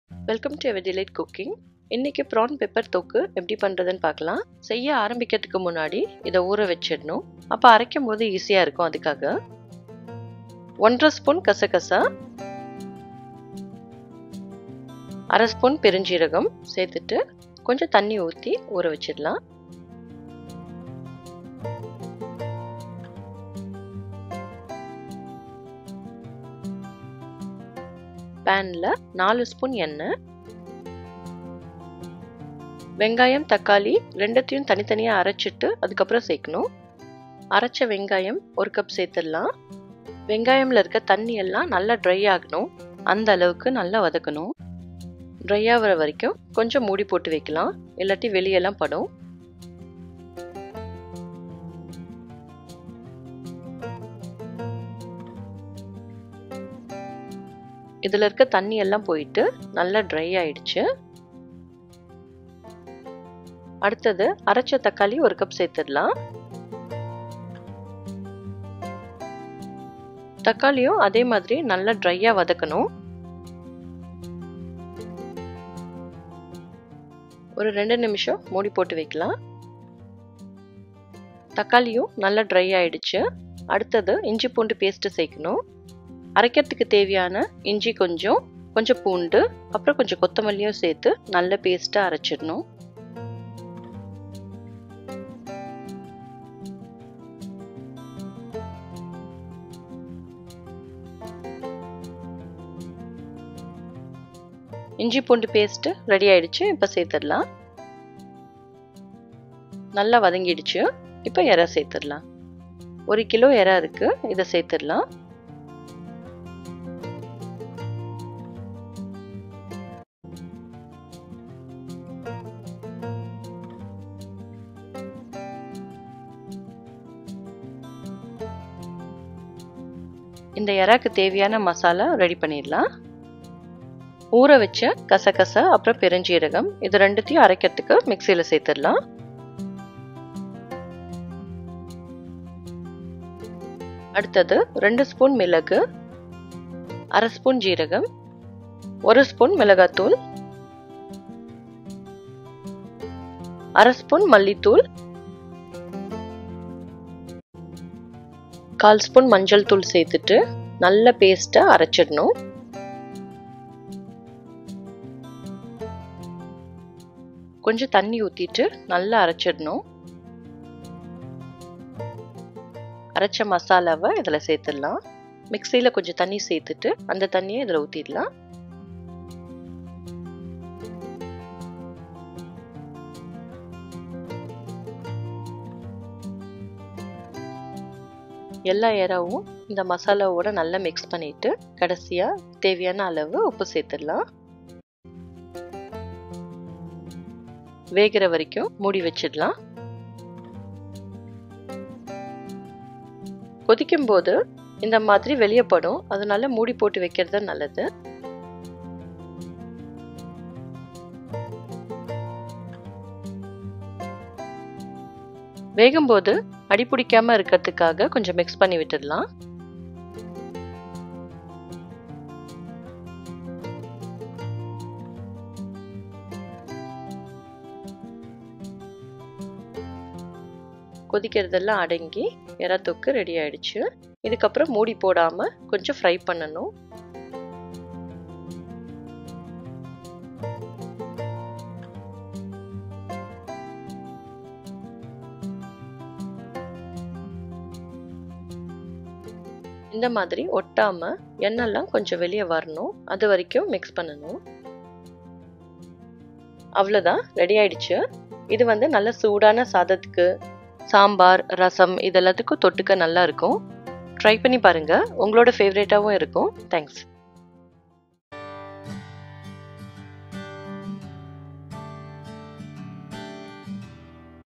Prawn Thokku| Prawn Pepper Thokku| Prawns Fry| Eral Thokku| Prawn masala| How to make Prawn fry
This Video is about how to make Prawn Pepper Thokku| Eral thokku a spicy and mouth watering recipe